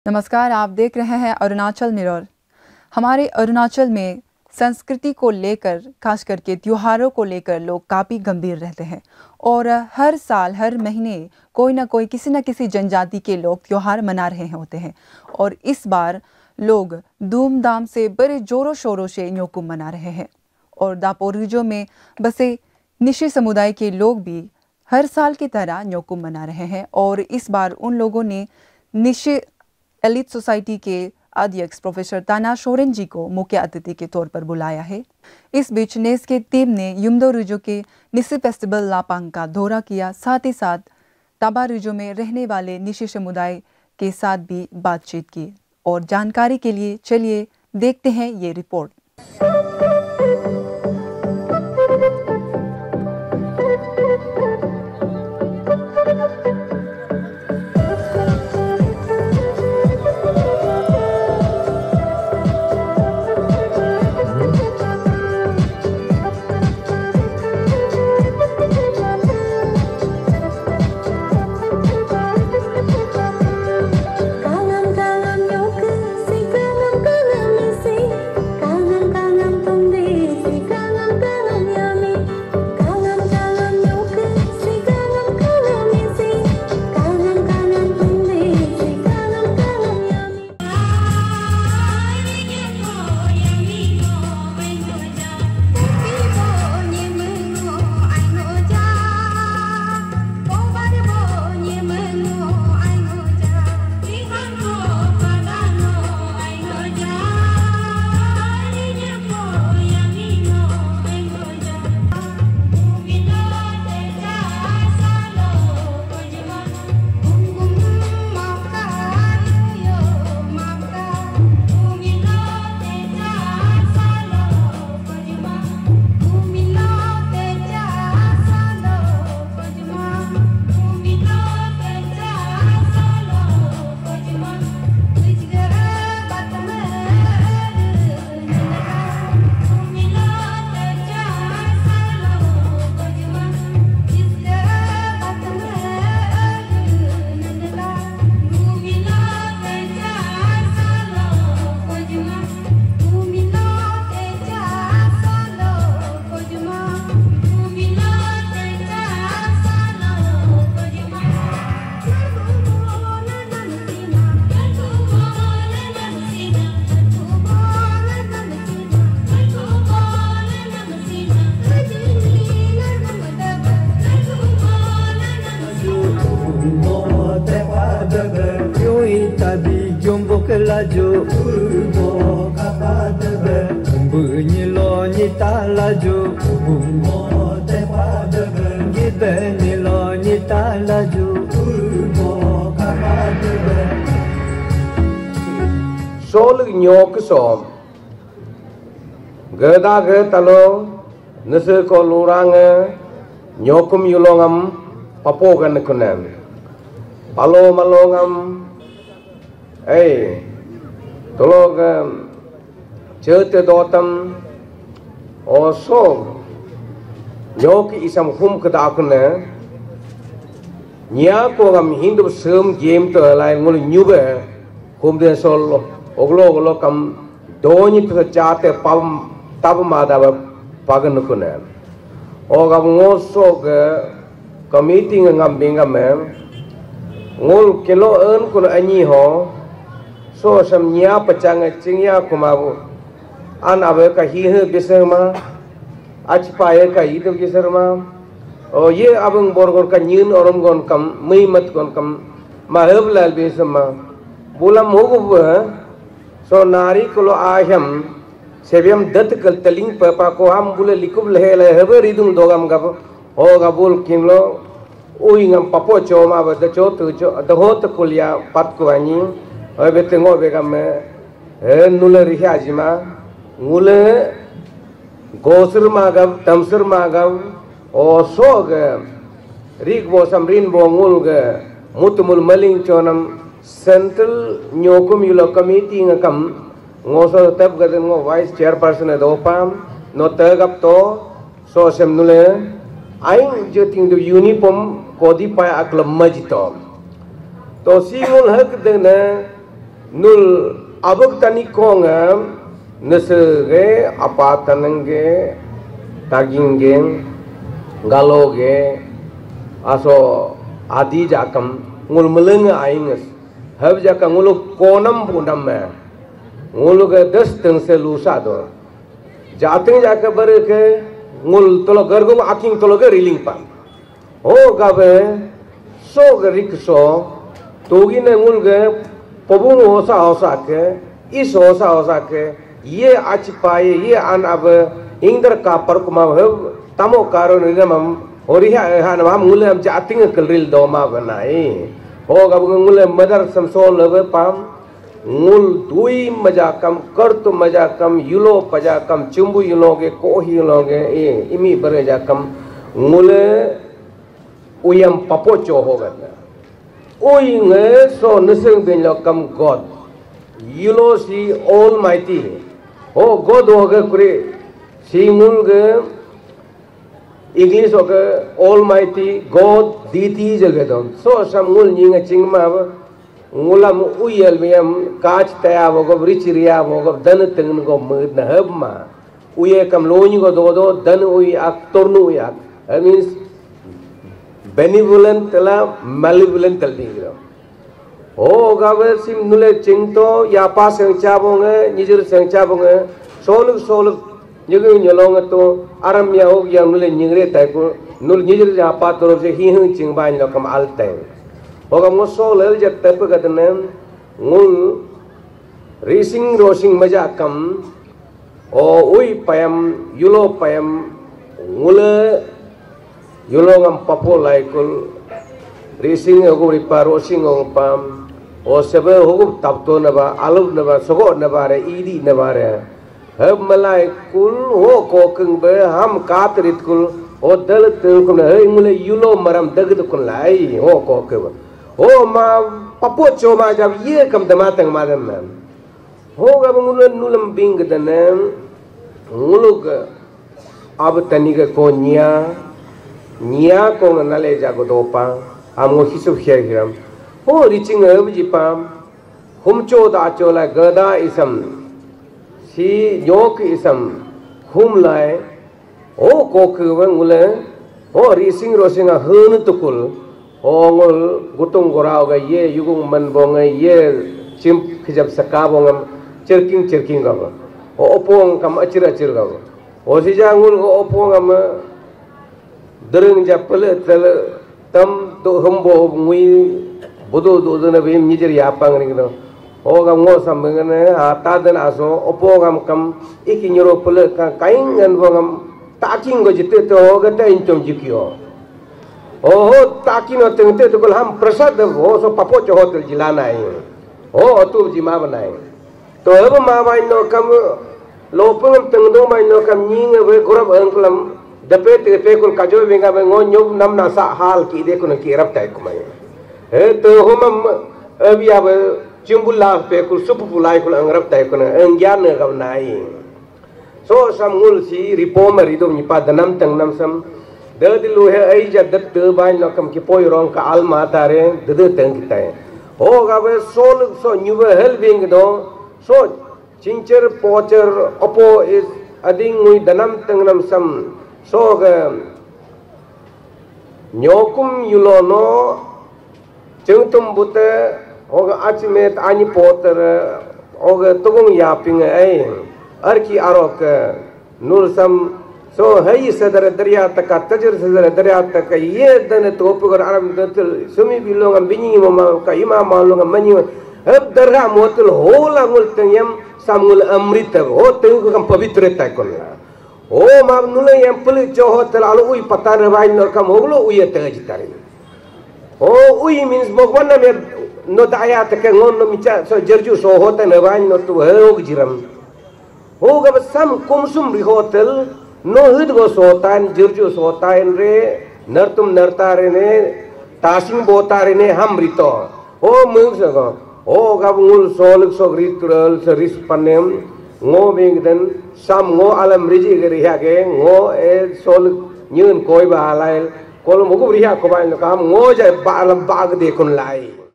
Nyokum Yullo celebrated at Daporijo with traditional fervor.
Nyokum Yullo celebrated at Daporijo by Nyishis residing in Upper Subansiri district amid the presence of NES President Prof. Tana Showren.
While attending the Nyokum celebration, NES team visited the Nyishi Festival Lapang at Yumdorijo and interacted with the community residing in Tabarij